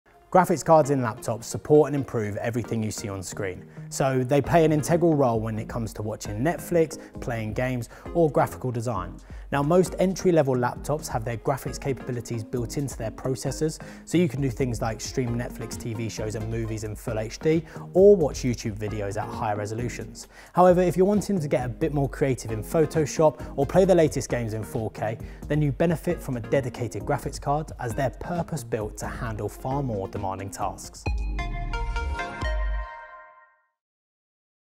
What is a graphics card?
Your laptop's screen performance relies on a good quality graphics card. This video explains why graphics cards are essential when choosing a new laptop.